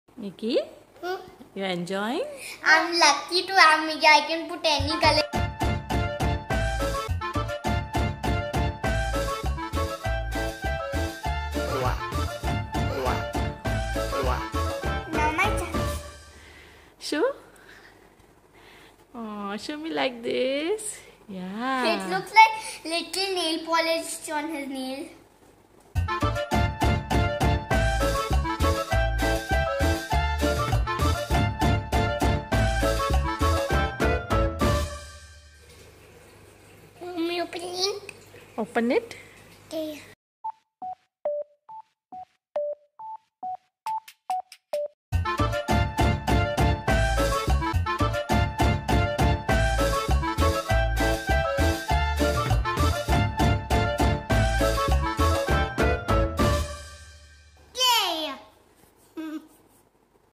Fun activity with Bindi || Activity to improve motorskill and concentration of toddler on
Fun home activites for toddlers on check out how to improve have eye coordination of your baby through Bindi activities.